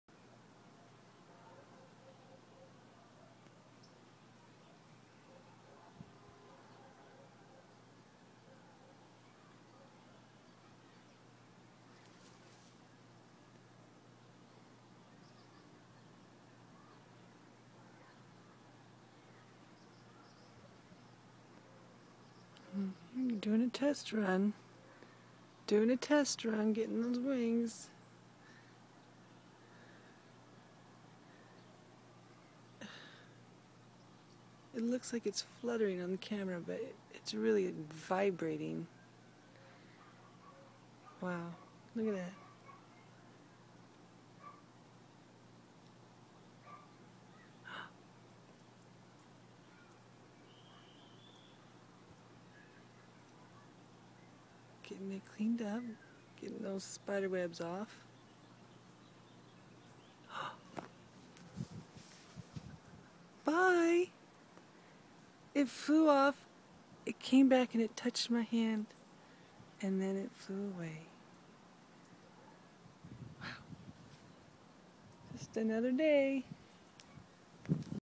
VIBRATING Rescued Sphinx Moth Lives Another Day! [HD]
Stuck in a cobweb on a windowsill, this moth didn't have a chance so I scooped it into my hand and went to release it outside.  I had to pull off bits of the cobweb and let the moth sit in my hand for a few minutes to collect itself.  Then it started vibrating in my hand, apparently doing a 'wing test'.  A few rounds of wing vibrations and cleaning its face and then the moth flew off, swooping back once across my hand and then off into the trees.